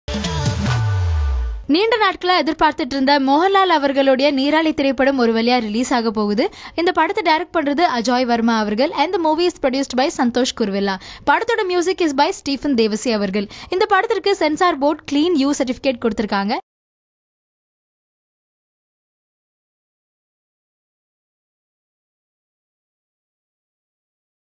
Mohanlal's Neerali All Set to Hit the Screen | First Frame
Mollywood Superstar Mohanlal will be seen on the silver screen through his upcoming venture Neerali directed by Ajoy Verma.